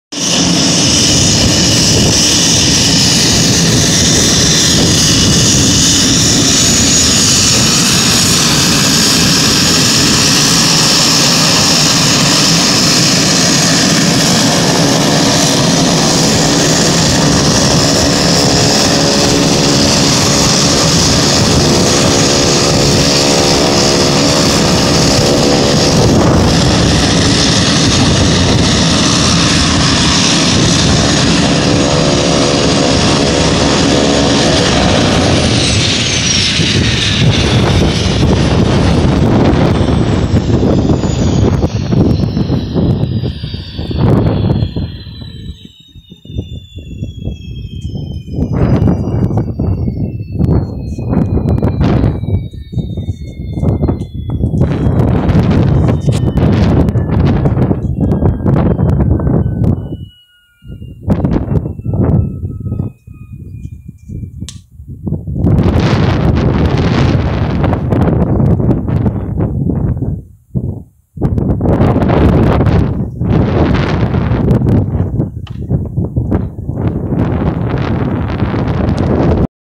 Pilatus PC-12 Turboprop Taxi & Shutdown at Princeton Airport (39N) On A Windy Day
Here is a 2012 Pilatus PC-12, N708AF, during taxi on the ramp at Princeton Airport (39N) in Princeton, NJ followed by engine shutdown shorty after arriving on a flight from Norwood, MA (KOWD) on a windy day. I then had a brief conversation with a passenger who got off the plane & one of the pilots who told me that their flight on this gorgeous aircraft today was a little bumpy. I knew it was because of turbulence. Enjoy the sight of a turboprop airplane  in HD.